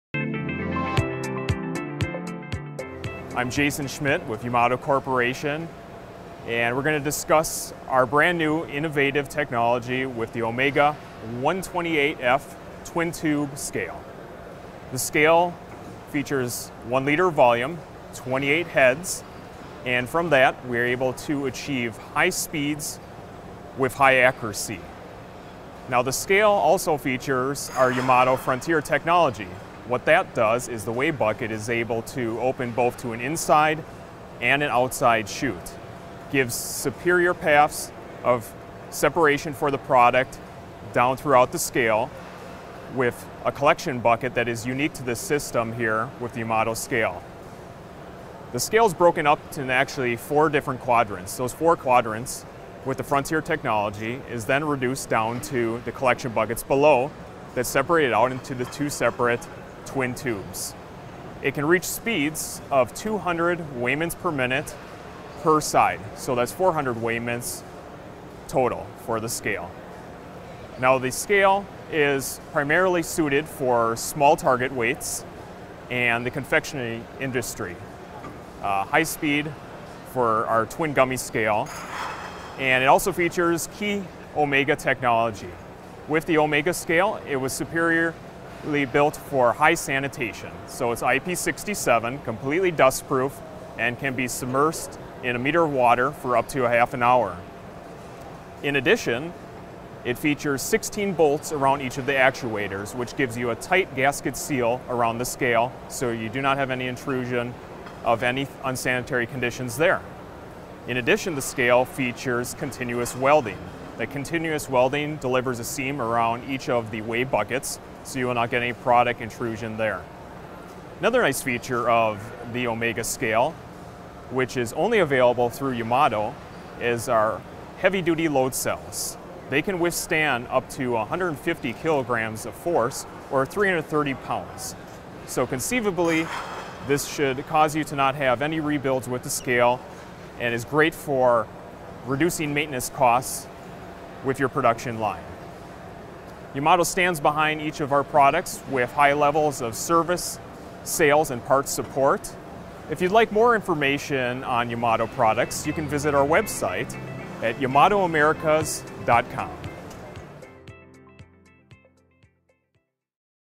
Twin Discharge/2-facher Abwurf, OMEGA Multihead Weigher/Mehrkopfwaage, Yamato Scale
Multipoint Discharge

From 1 up to 6 Discharge Gates Yamato Multihead weigher can fill tray lines, cup lines, HFFS, VFFS, rotary packing machines and other similar packaging applications. Multipoint Discharge allows increase of production.

Mehrfacher Abwurf
Mit dem OMEGA Frontier Konzept wird die Genauigkeit mittels des sogenannten gleichzeitigen Kombinationsfindungskonzepts erhöht. Hier werden bei der Ermittlung des exakten Zielgewichts simultan mehrere Kombinationen errechnet, wodurch die Genauigkeit der einzelnen Kombinationen steigt. Der Anteil der eingesetzten Rohware sinkt auf ein Minimum (Reduzierung des „Give-Away“).